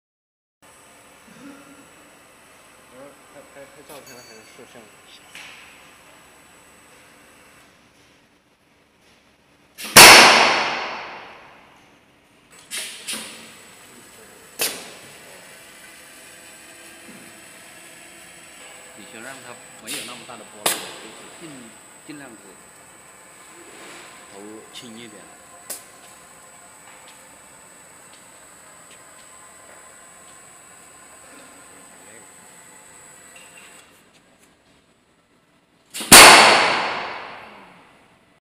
Labtone High Speed Mechanical Shock Test Equipment
High Speed Shock Test Equipment
Max. Specimen Weight: 30kg
Table Size: 300 x 300 mm
Acceleration range: 20-10000g (bare table) 35000g (with DMSA)